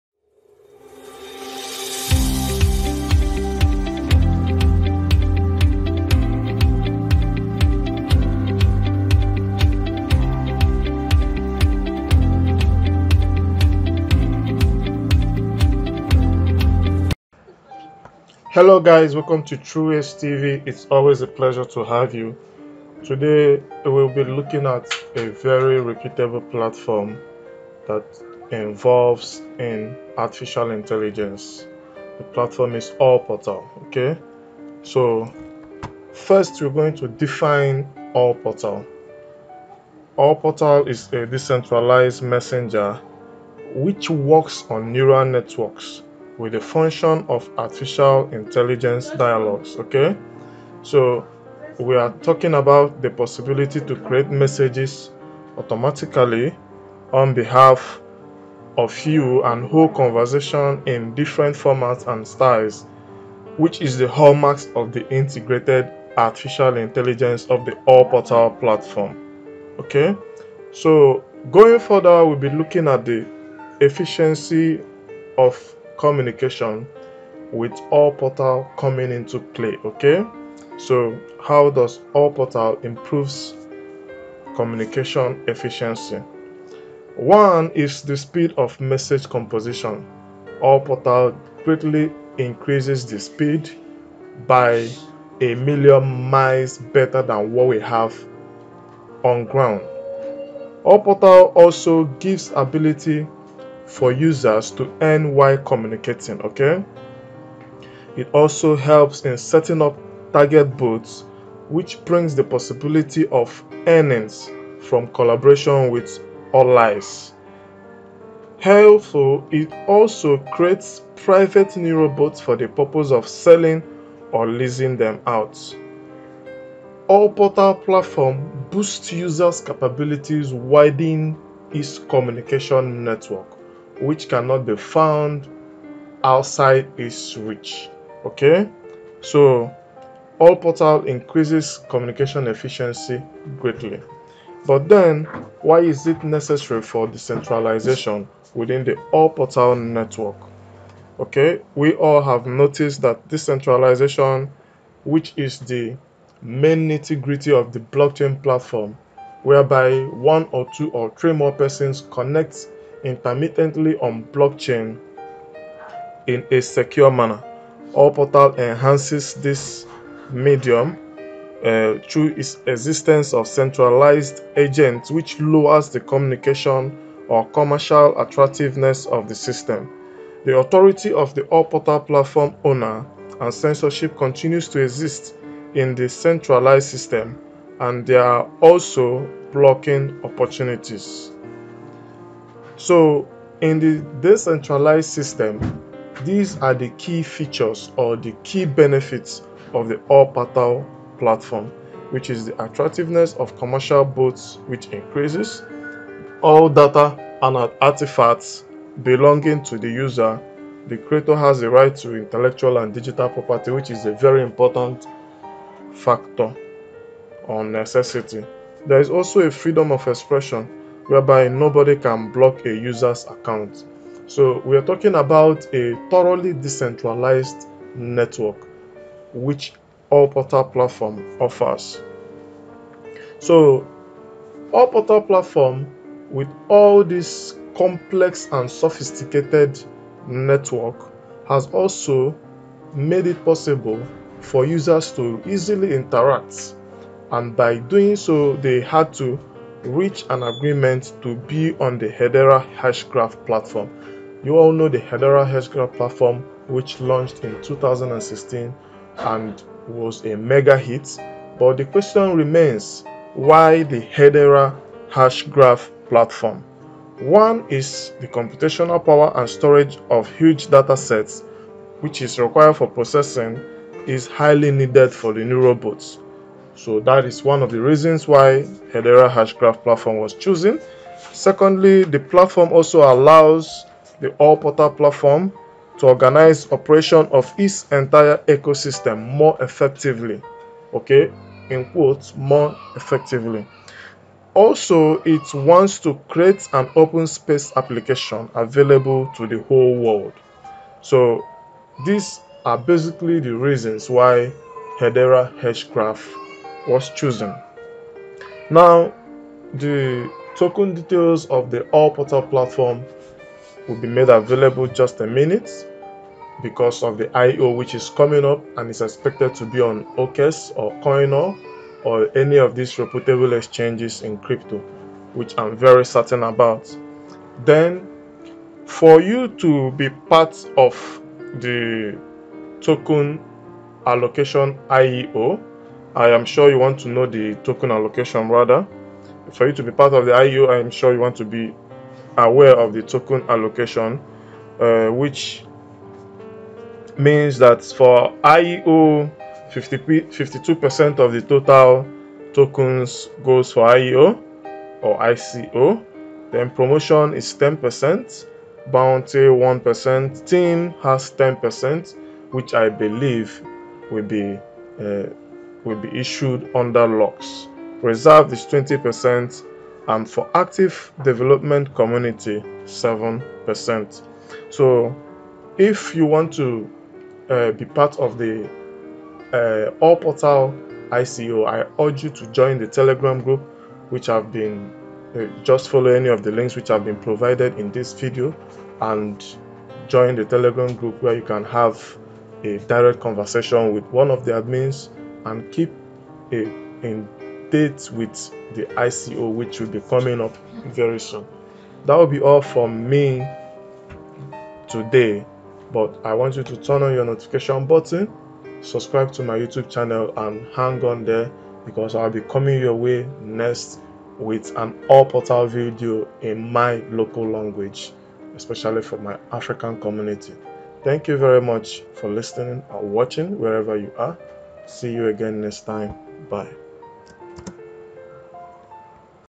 OlPORTAL: Ushering Instant Messaging Users to A New Realm
OLPORTAL is a messenger on neural networks with the function of Artificial Intelligence dialogue (Ai). The hallmark of the integrated Ai is the possibility to create messages automatically on your behalf and hold the conversations in different formats and styles. For this purpose, we create OLAI neurobots with different characters. Soon you will be able to create them by yourself.

Eth: 0x8e554fC6D8B03E9B5087863a551E40AFB72896BC